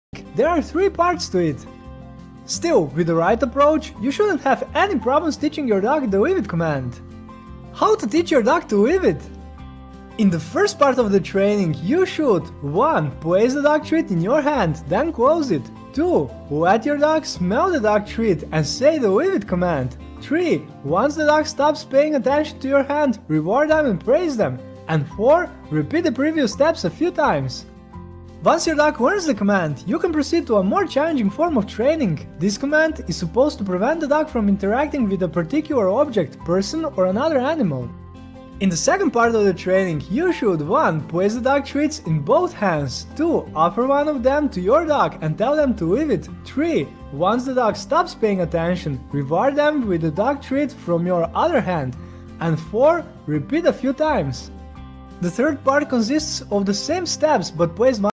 SHOTS How to teach your dog "leave it" command
Who Needs This Secret to using your dog's natural intelligence to stop bad behavior.